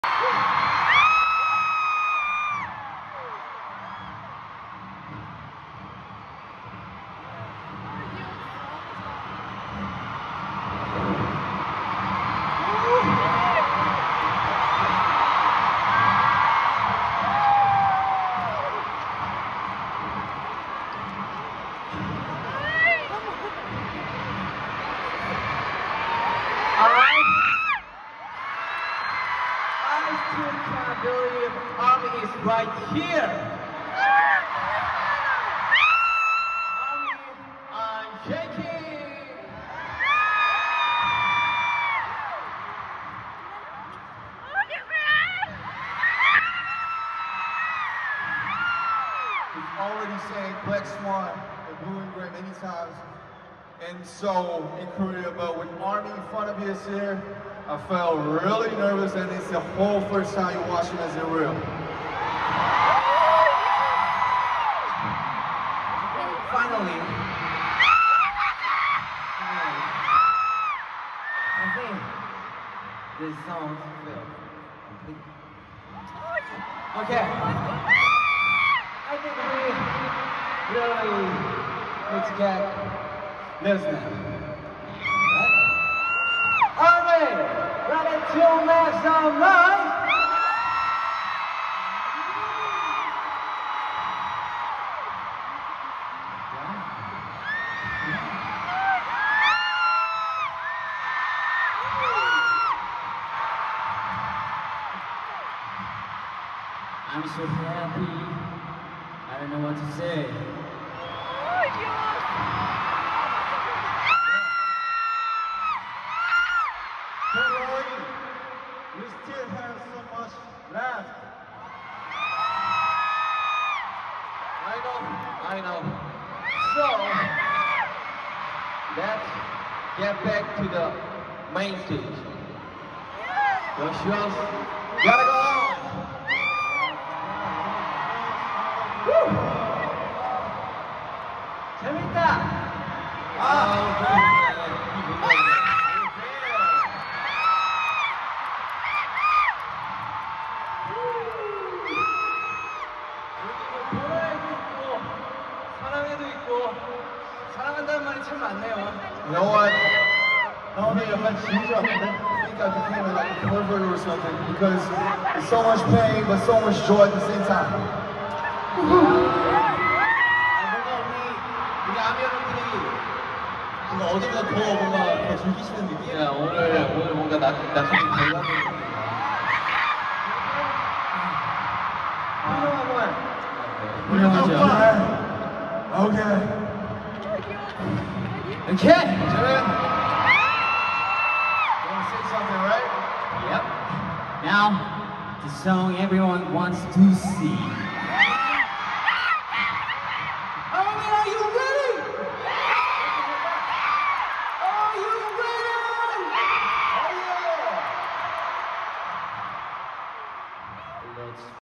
BTS Talking With ARMY - PTD DAY 2
BTS Talking with ARMY / Permission to Dance on Stage in Los Angeles / 211128 / PTD on Stage D-2